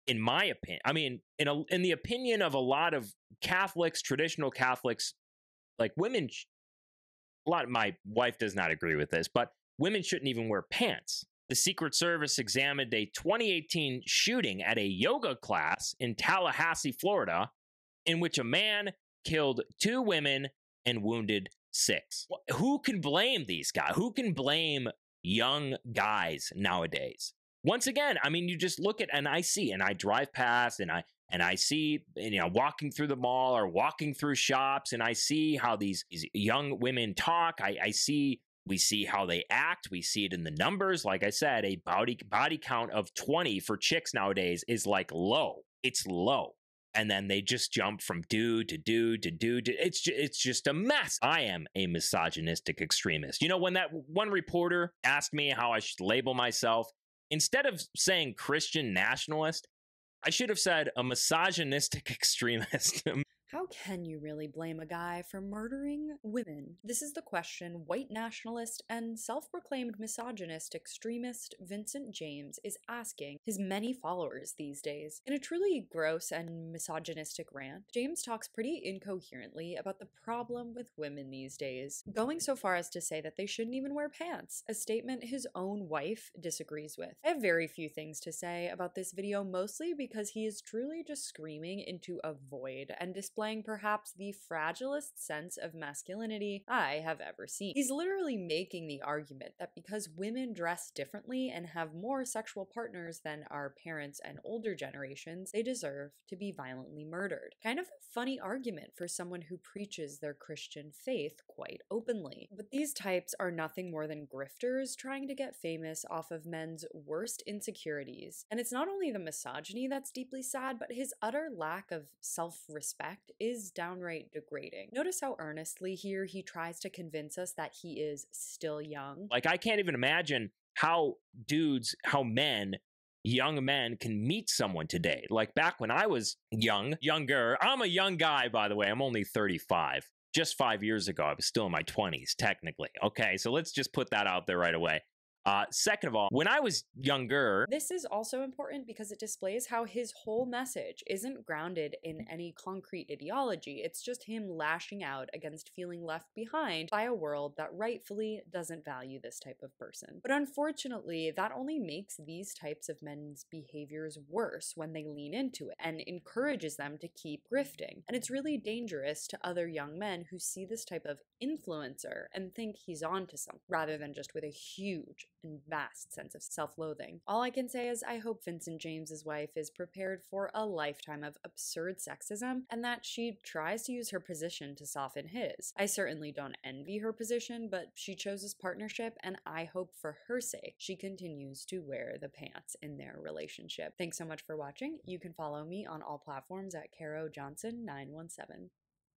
White Nationalist DEFENDS Attacker In Misogynistic Rant
Christian nationalist Vincent James goes on an unhinged rant, claiming himself to be a
"misogynistic extremist."

Caroline Johnson breaks it down on Rebel HQ.